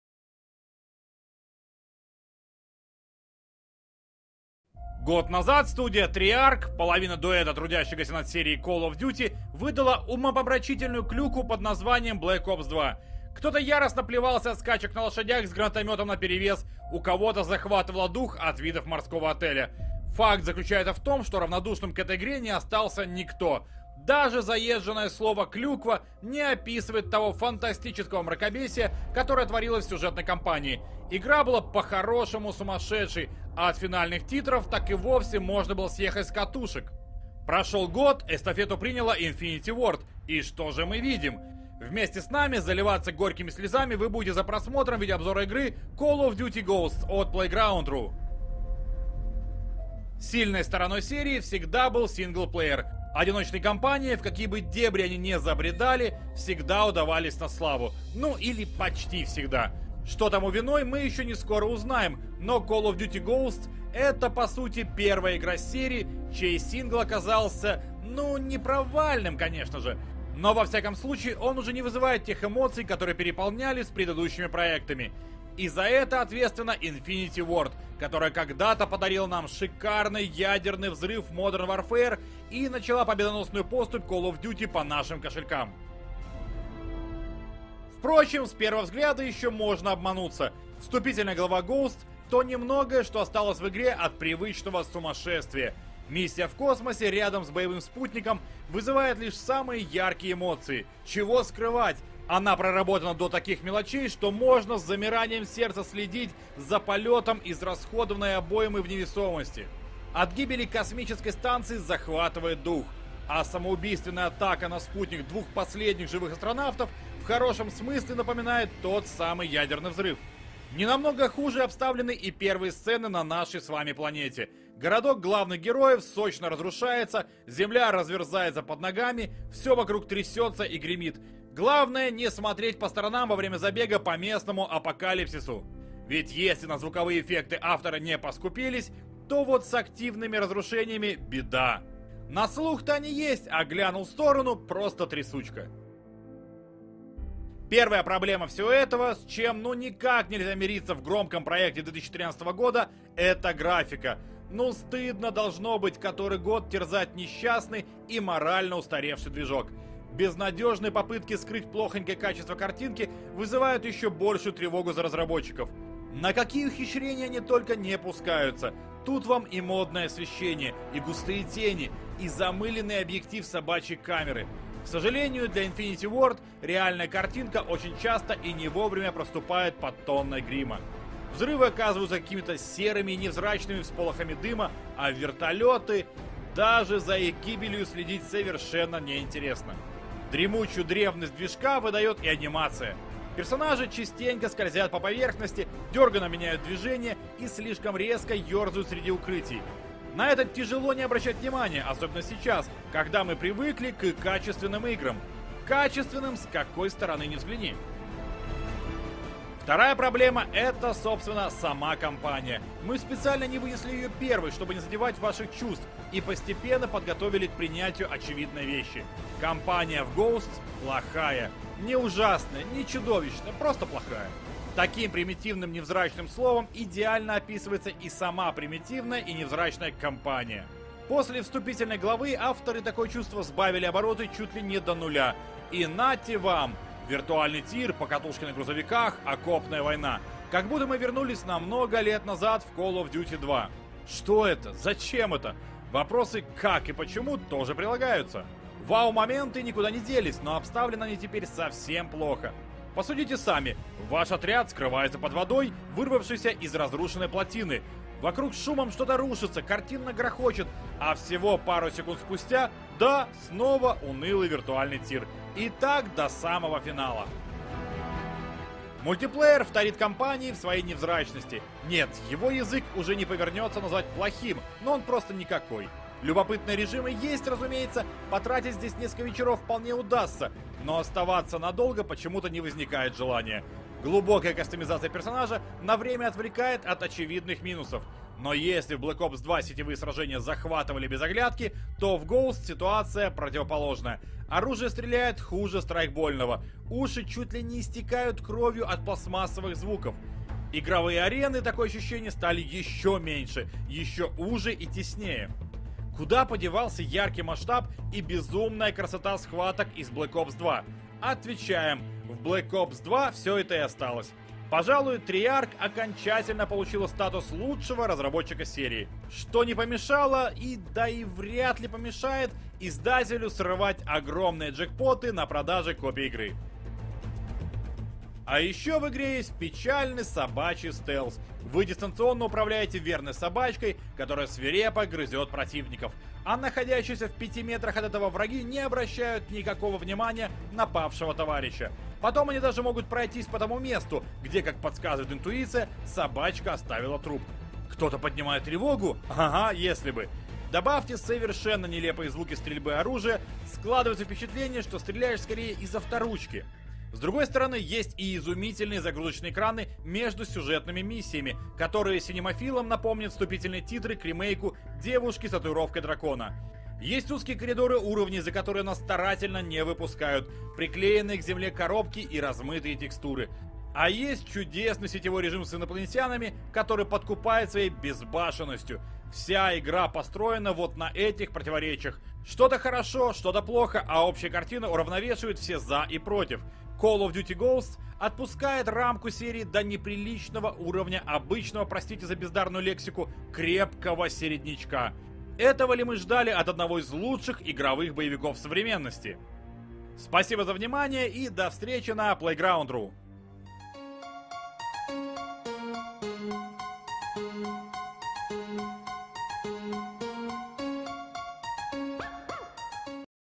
Видеообзор Call of Duty: Ghosts. Собачий вальс
К некоторым вещам привыкаешь настолько, что заранее готовишь соответствующую реакцию. Вероятно, именно с такими мыслями все ждали очередную часть Call of Duty, над разработкой которой трудилась в этот раз Infinity Ward.